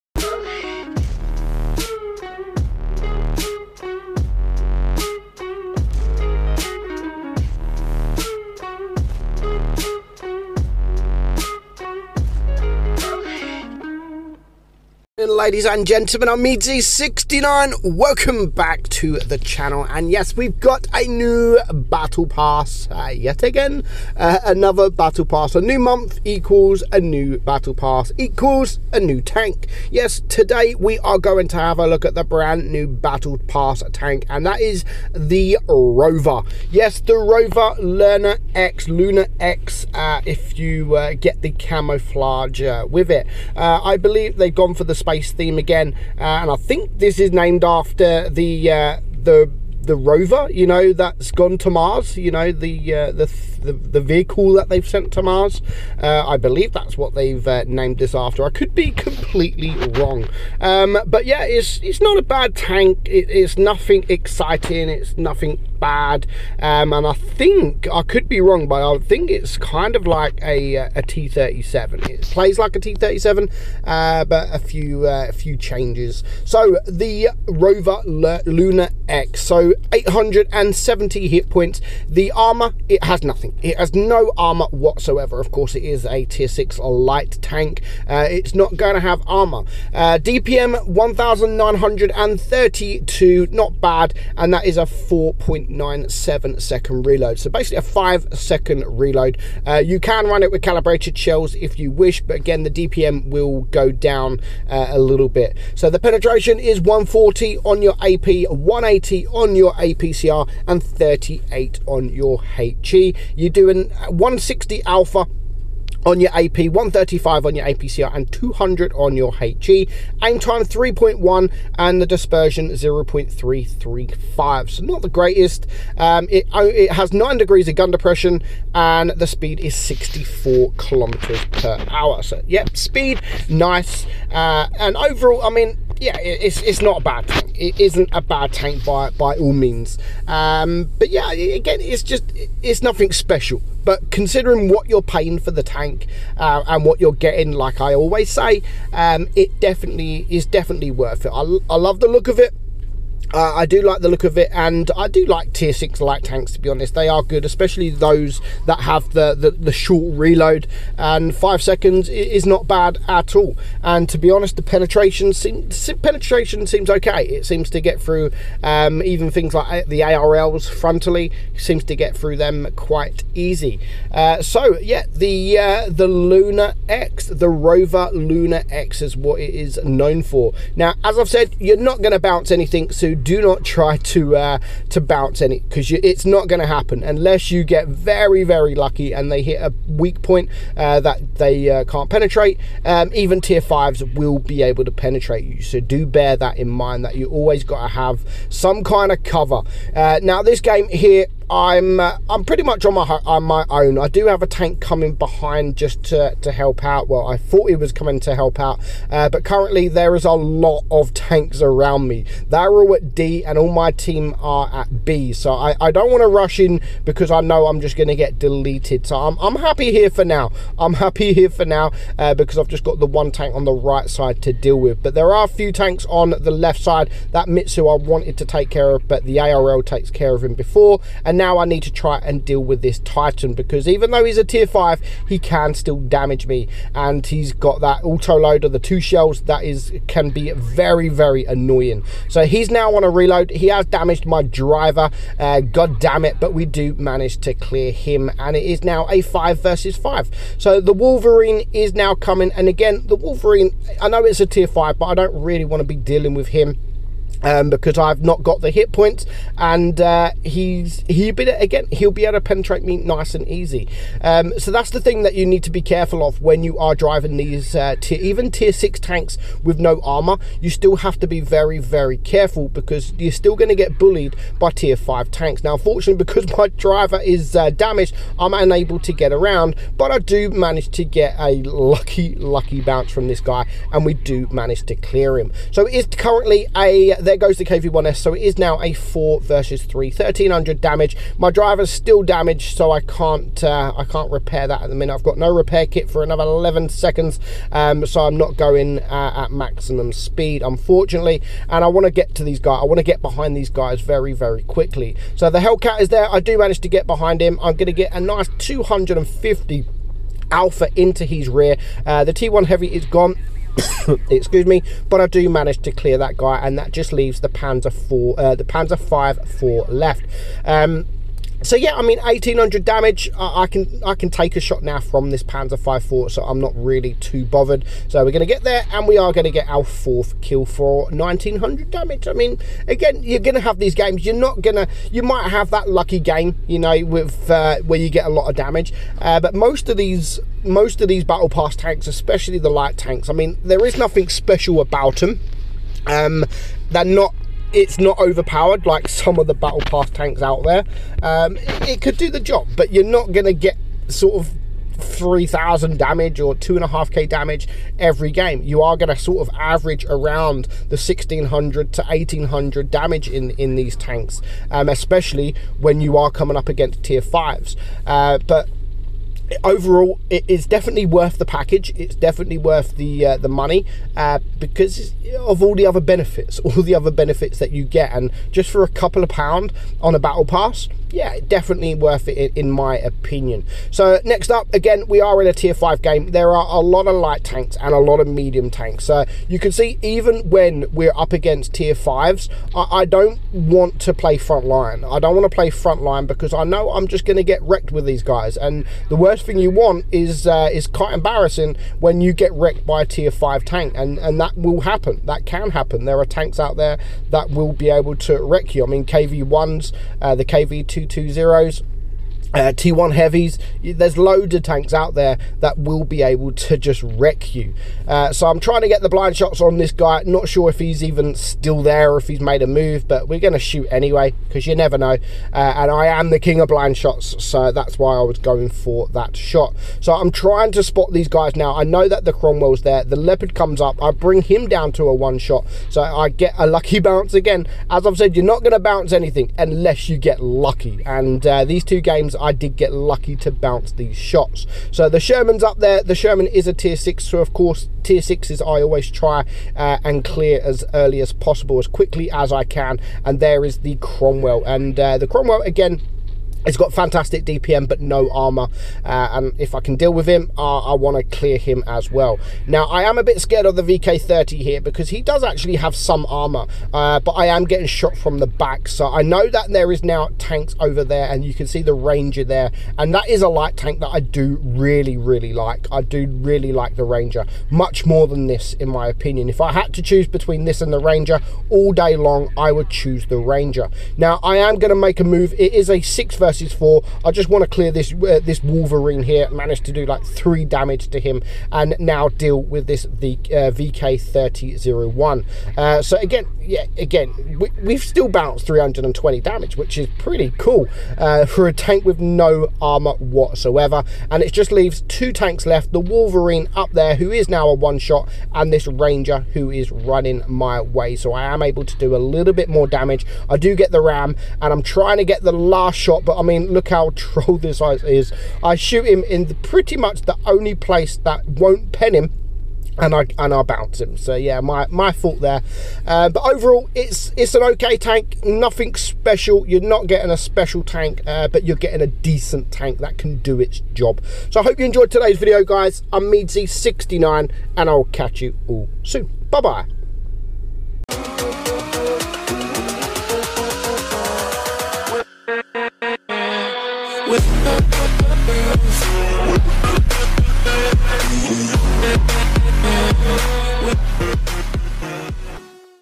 The Rover LunarX
in today's video we will be playing the new August Battlepass tank, The Rover. talking about its stats and if it's worth buying the Battlepass